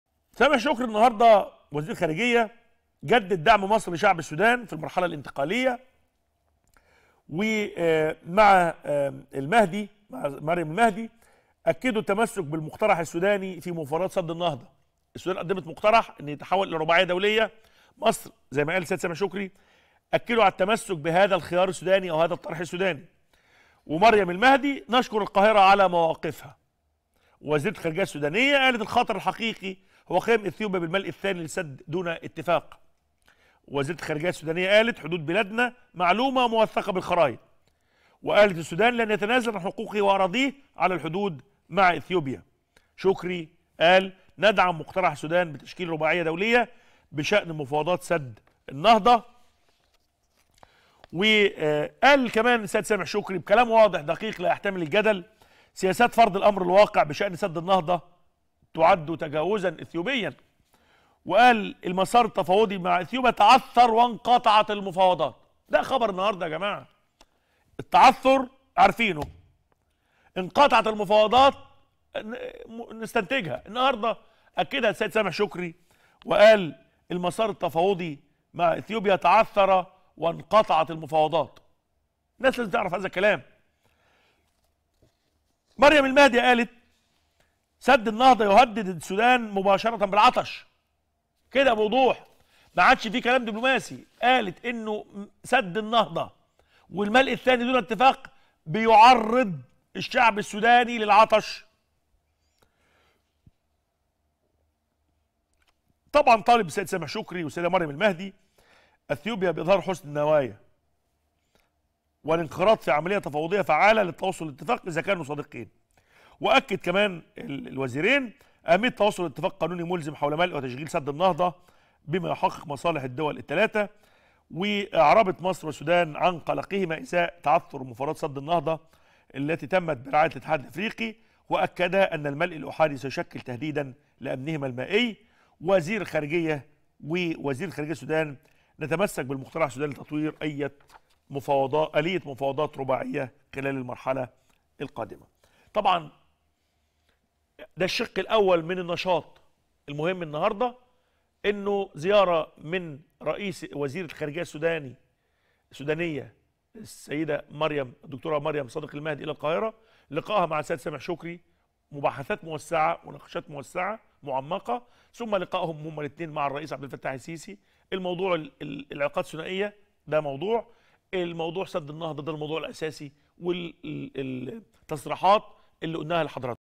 إثيوبيا تعرض السودان للعطش.. تصريحات قوية من وزيرة الخارجية السودانية أثناء زيارتها لمصر
الثلاثاء بتاريخ 2-3-2021